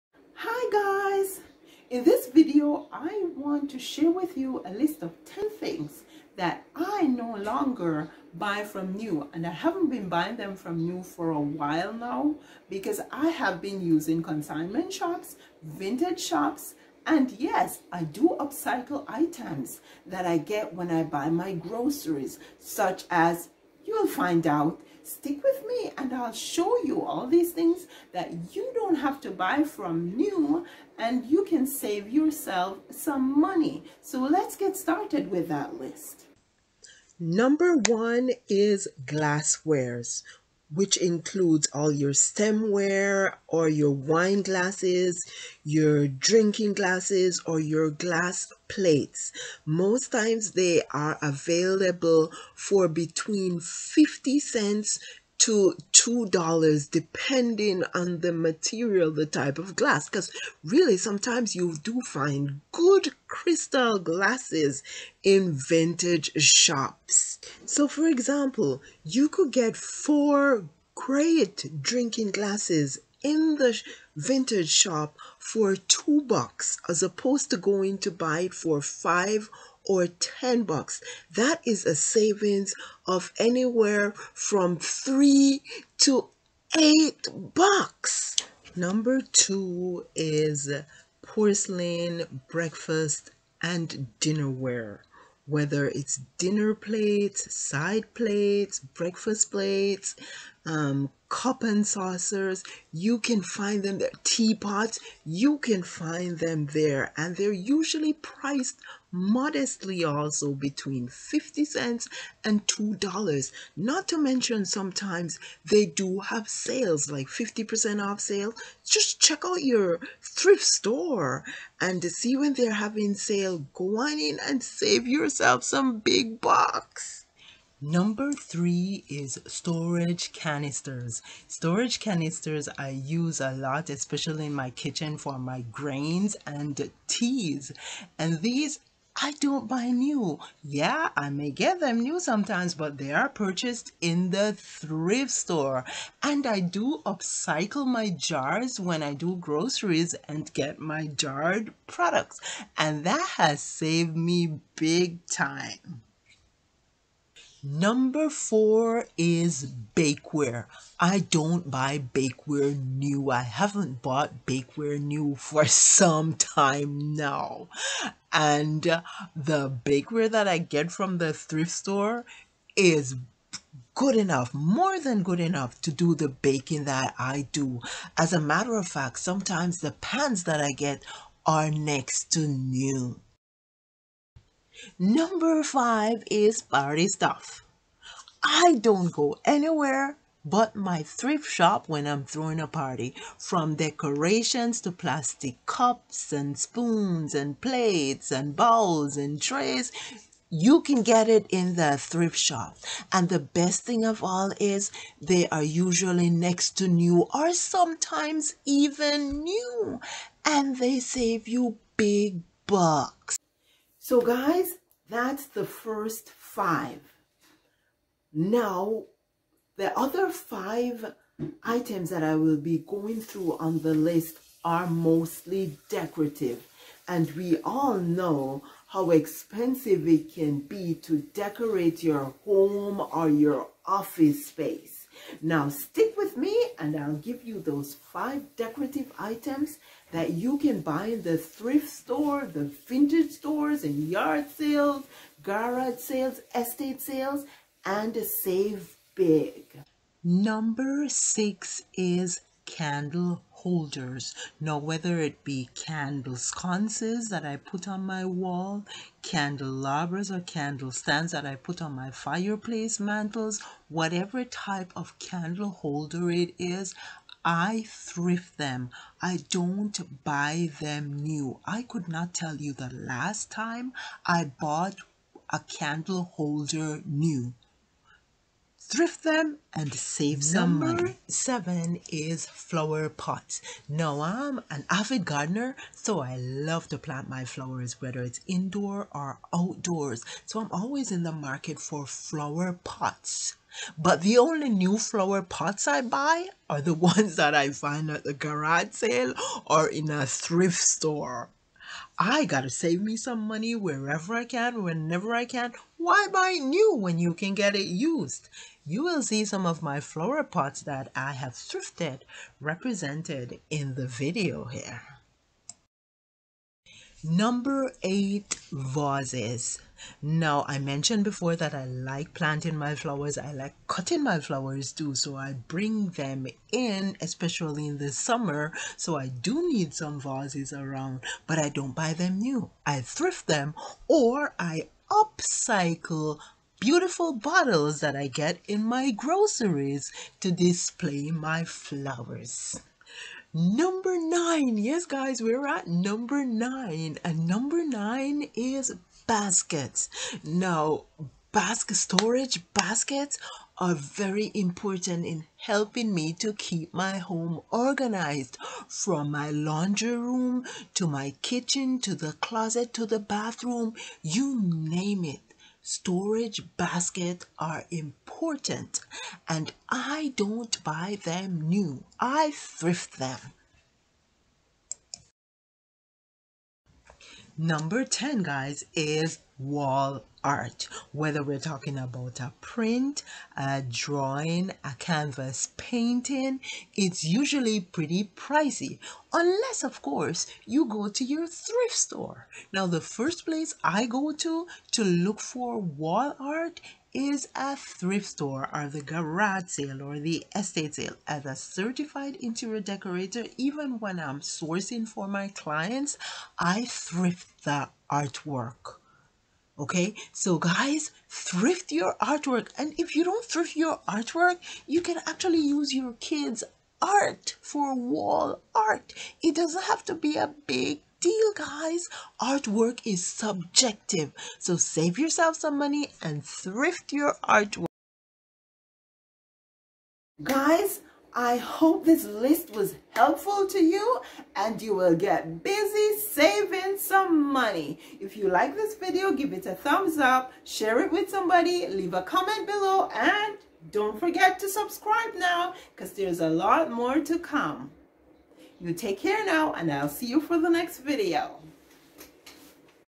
STOP Wasting Money on These 10 Things!
For real, never buy these 1o things new!
In this video I share with you 10 everyday household items to save big on! There is no need to ever buy them new ever again. I have been thrifting them. upcycling for replacements or just foregoing them and have save myself and family big bucks, especially since they are everyday items that are liable to break or go bad and have to be replaced somewhat frequently!